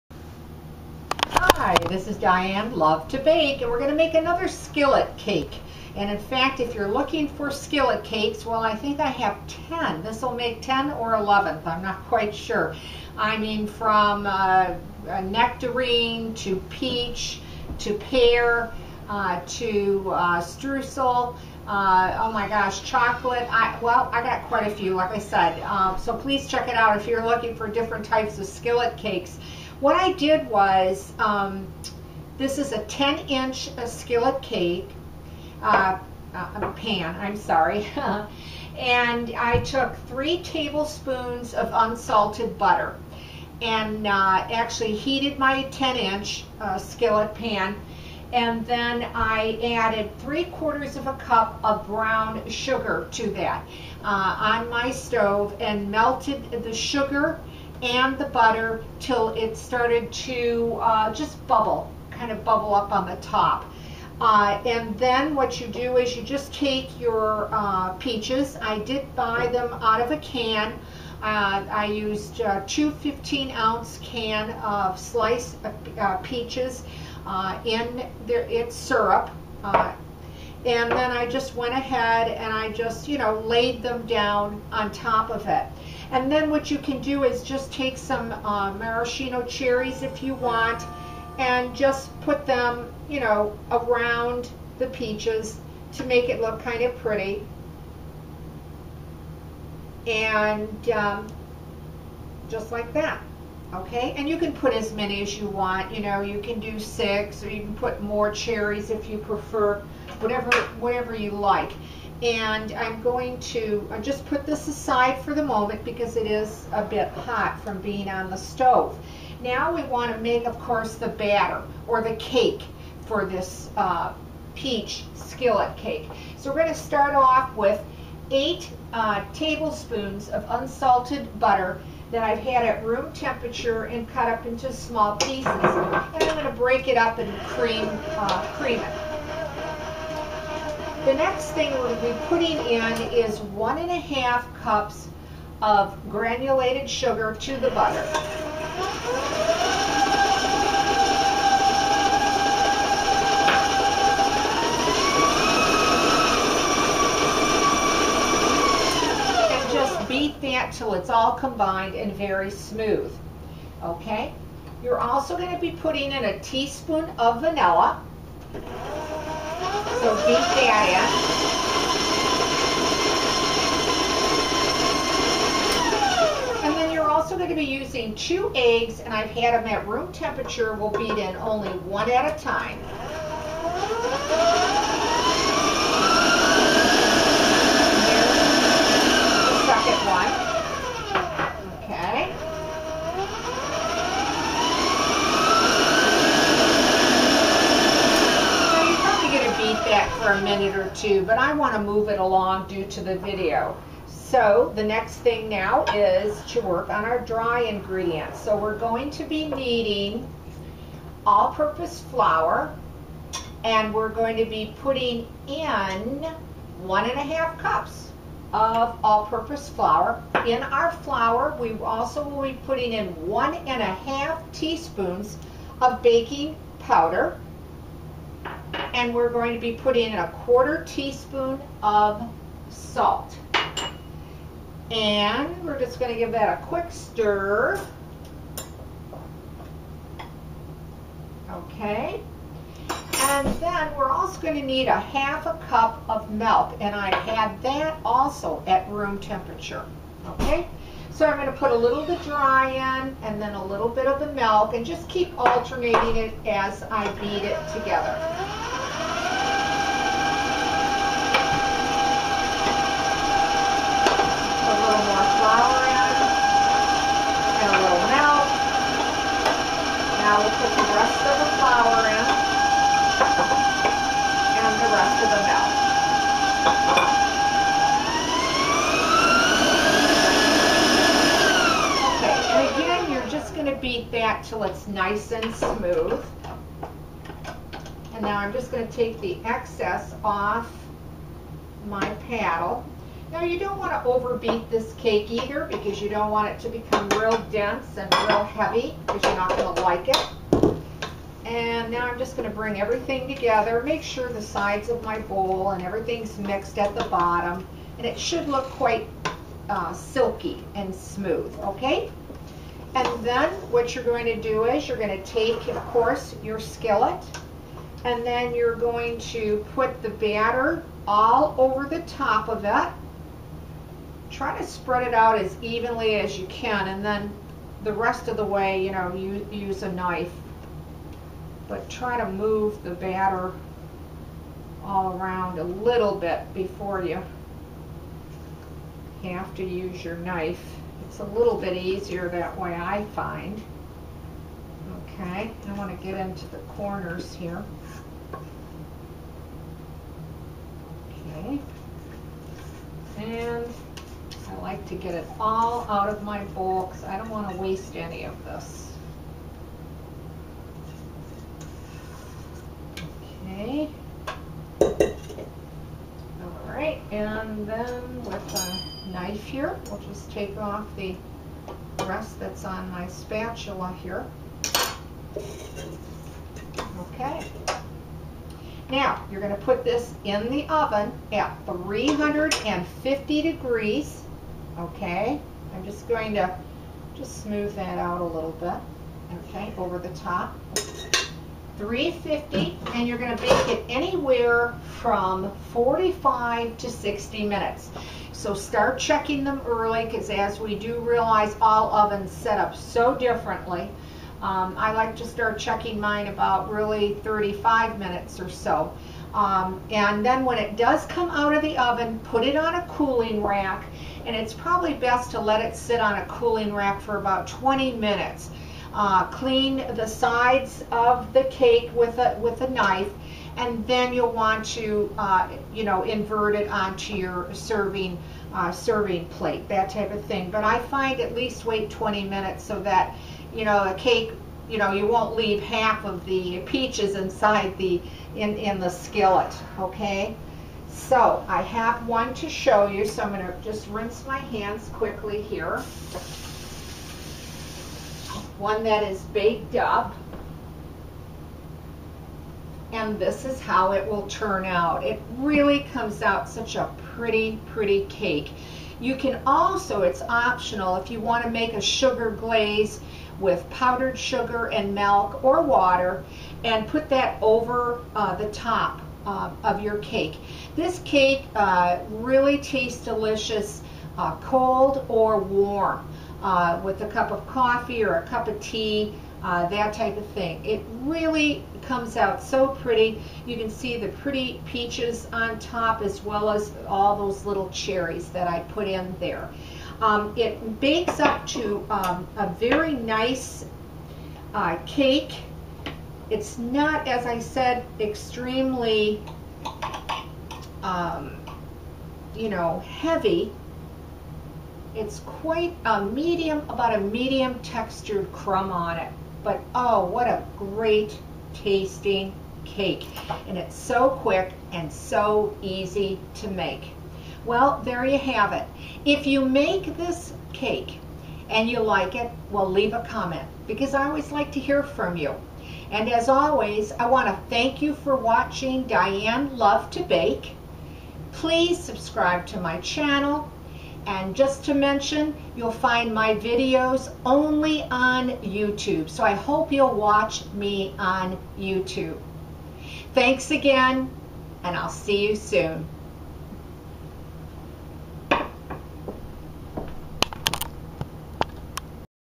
Skillet Peach Cake by Diane Lovetobake
The video and photo is owned by Diane Lovetobake shown on YouTube only . No Pinning, posting or copying.
3 Tablespoons unsalted butter
3/4 cup brown sugar
2 15 oz can peaches in syrup
Melt butter and brown sugar in skillet till it melts in a 10" skillet
Arrange peaches in skillet a few cherries  set it aside
8 Tablespoons unsalted butter
3/4 cup granulated sugar
1 teaspoon vanilla
2 eggs room temperature
1 1/2 cups all-purpose flour
1 1/2 teaspoon baking powder
1 /4 teaspoon salt
1/2 cup milk room temperature
Beat butter and sugar till smooth add vanilla beat in eggs
Add dry ingredients slowly and alternate with milk
Don't over beat cake batter
Pour batter over peaches evenly
Bake  at 350 for 45-60 minutes
Place hot skillet on cooling rack for 20 minutes
Place a plate over top of cake and invert upside down.
Optional Make  powder sugar glaze with milk or water to desired thickness.
Serve warm or cold